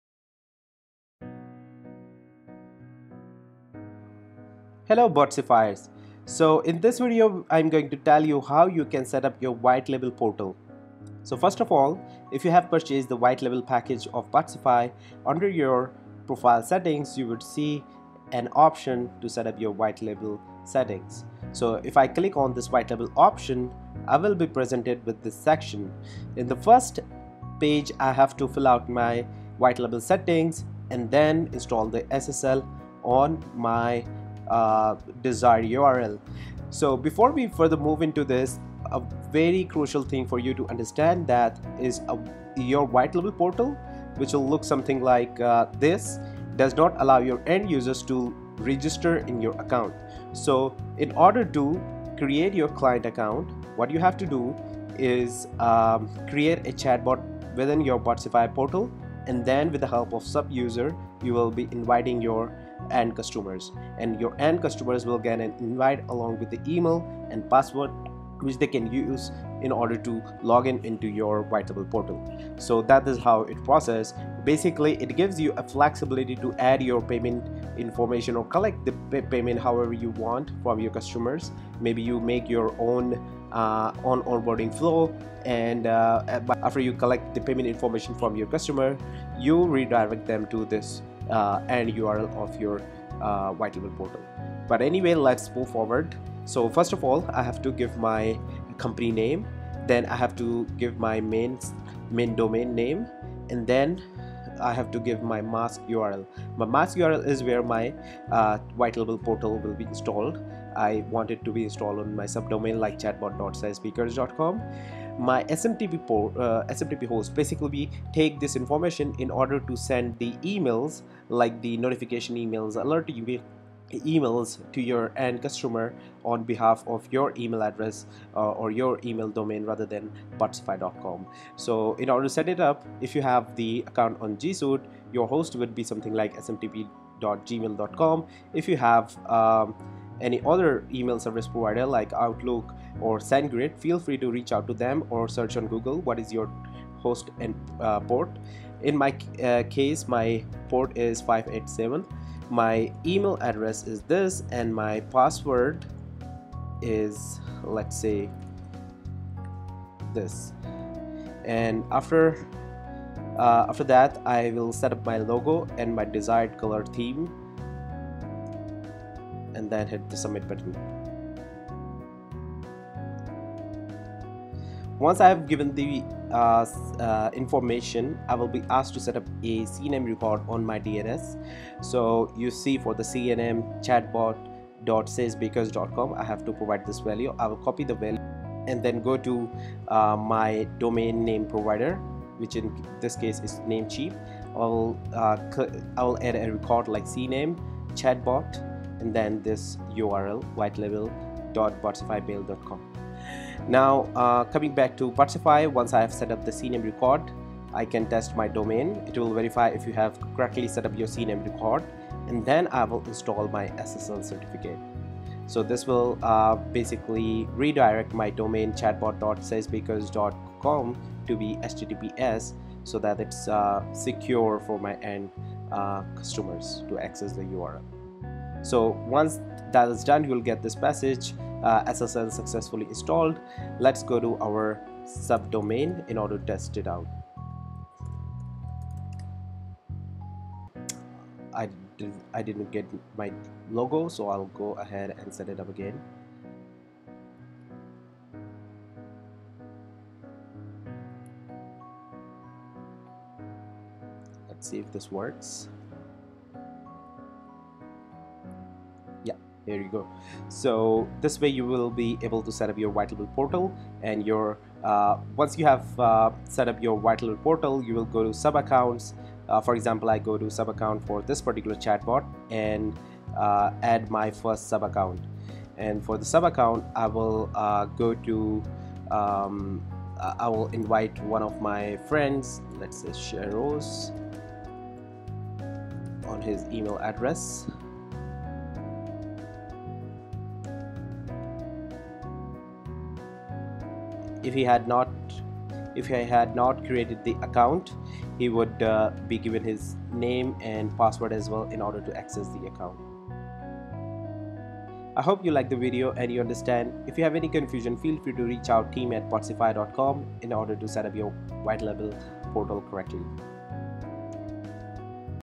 How to setup Whitelabel Portal with Botsify
In this video we explain how you can setup your whitelabel portal for your end customers.